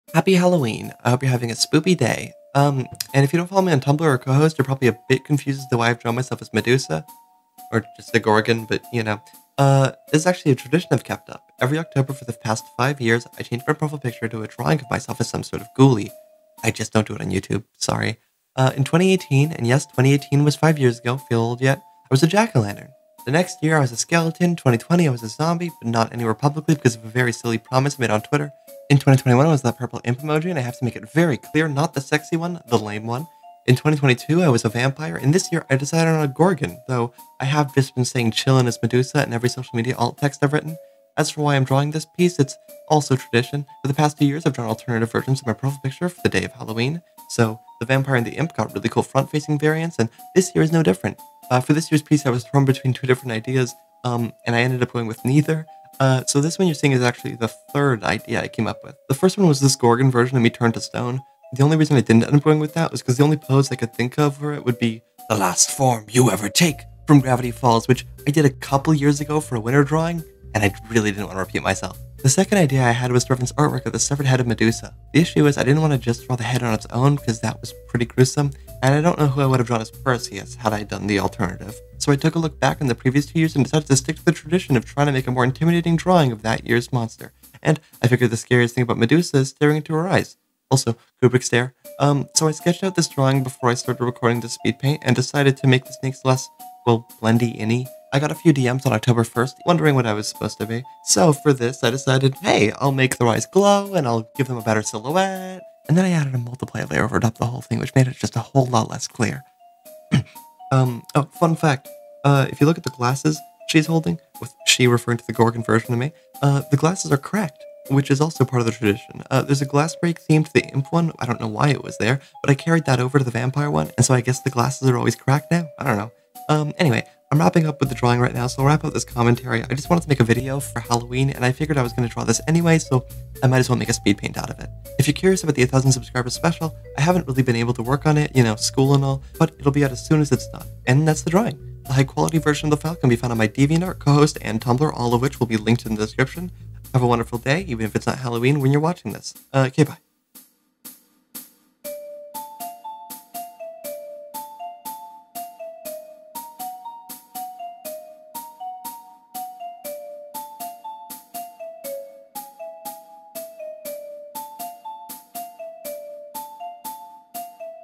Medusa Chillin (Halloween Speedpaint)
tbf it's not much of a speedpaint
the screen is covered by graphics 50% of the time